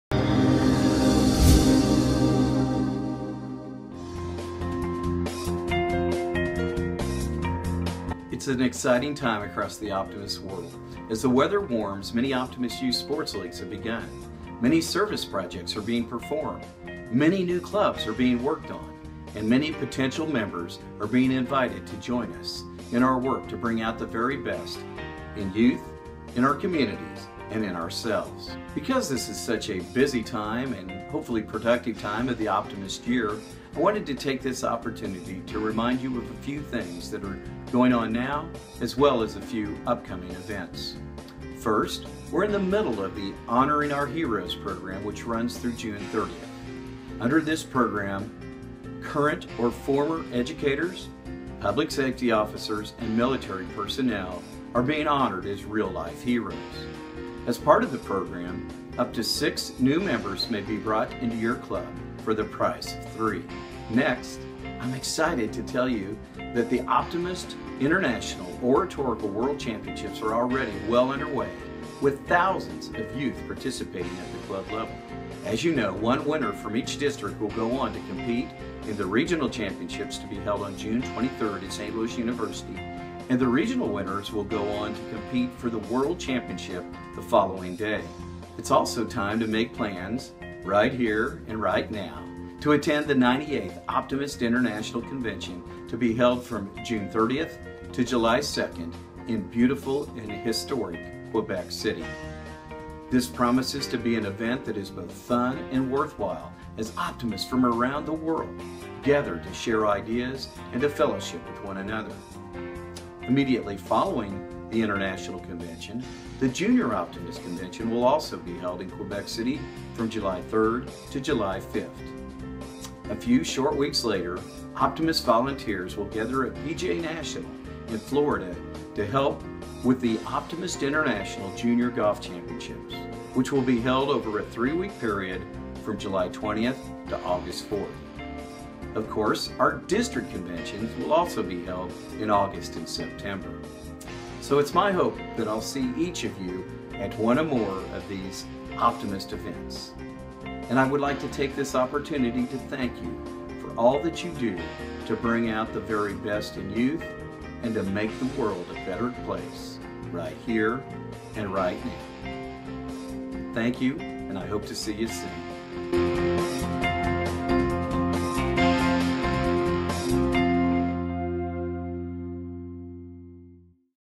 Dave Bruns - La ligne directe Mai 2016
Le président international Dave Bruns nous encourage à participer au programme « Rendre hommage à nos héros » et nous annonce les évènements à surveiller cet été.